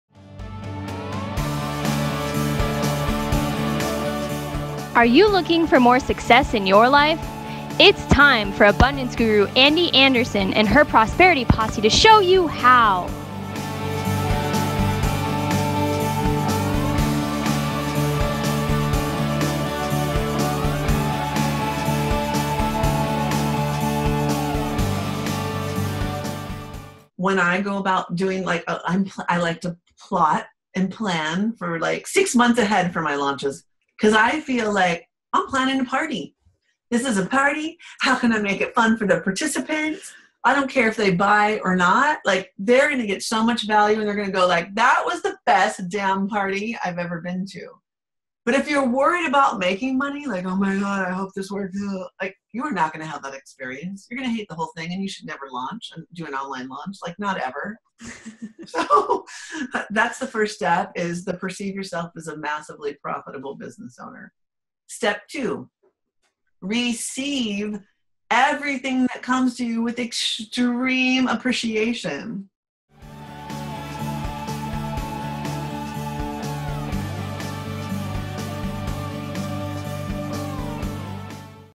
The art of product launches with Jeanna Gabellini & Ande Anderson
Watch all of Jeanna Gabellini's interview and 23 other entrepreneurial experts in the free BE, DO SUCCEED Global Online Conference.